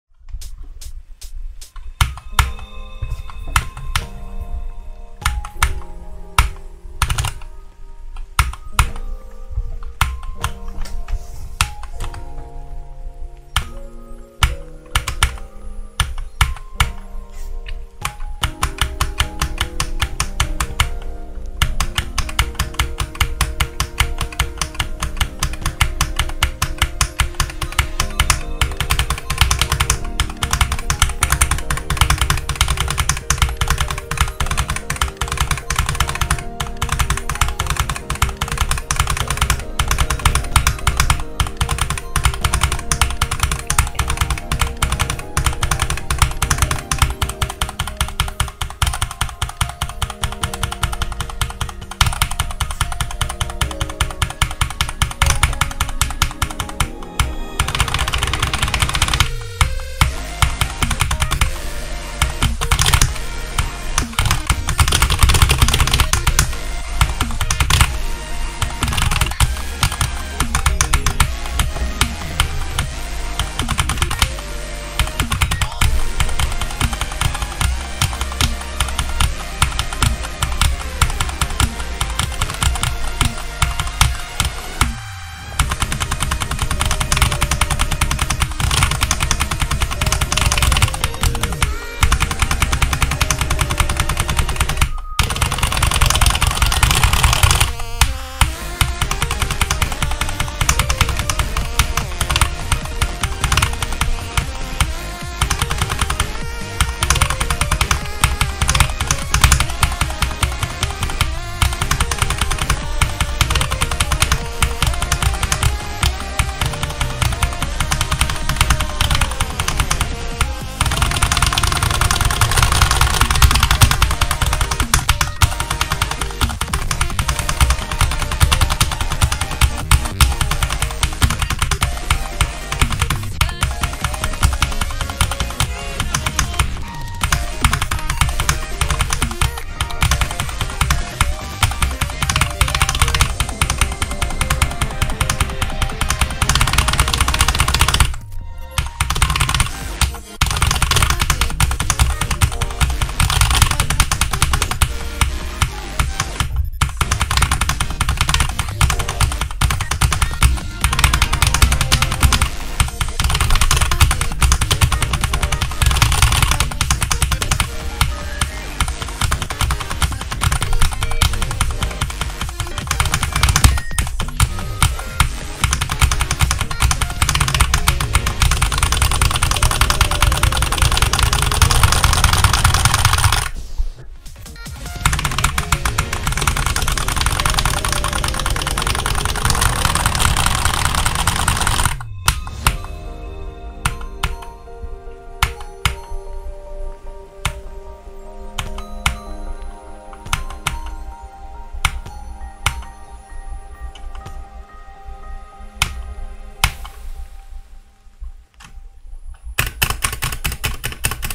안밖 유저가 밖안을 쓰는 방법
3:03 사망
못깸

포럼 레벨: 20.1

체감 레벨: 20.1

키보드: COX CK87 ( 크림블루, 갈축 )

키뷰어: KeysPerSecond v8.4